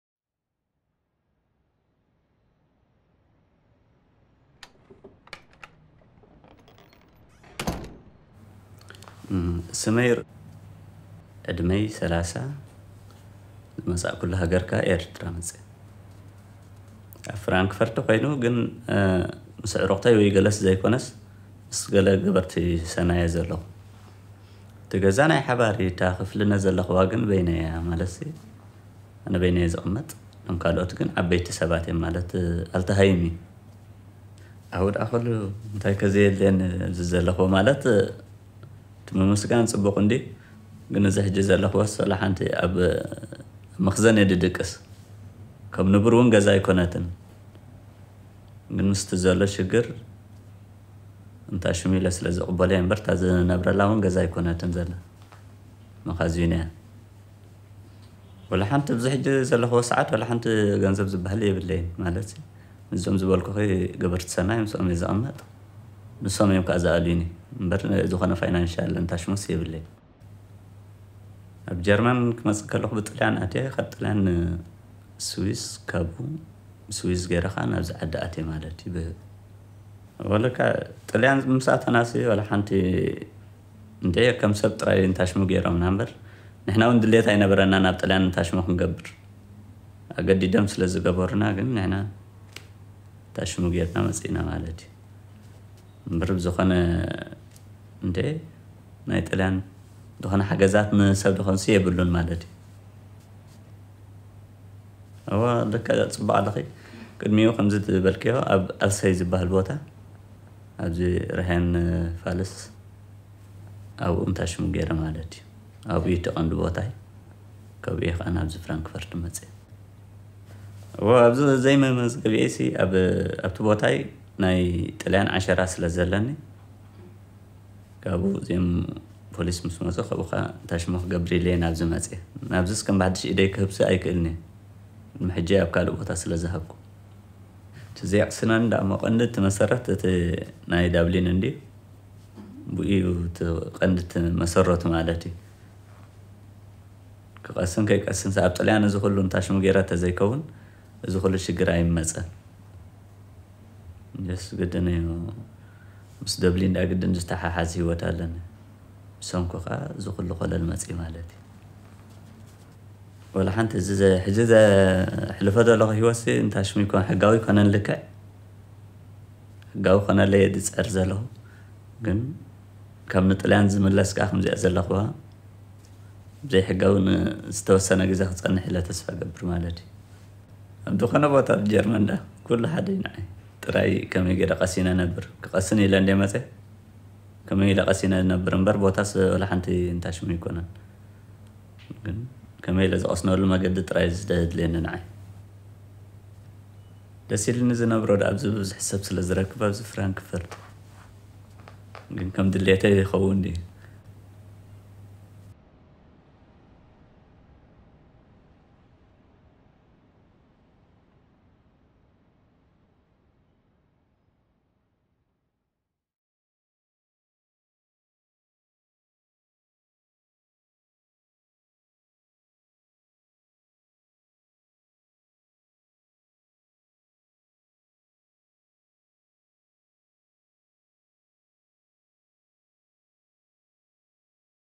Wohnen ohne Anmeldung: R., 30, aus Eritrea, erzählt
Der Film entstand im Rahmen der Tagung "Wohnen ohne Anmeldung", begleitend zur Ausstellung MAKING HEIMAT. Germany, Arrival Country.

Weitere Informationen unter: goo.gl/MXaYJ9

Ein Film von Matthias Lawetzky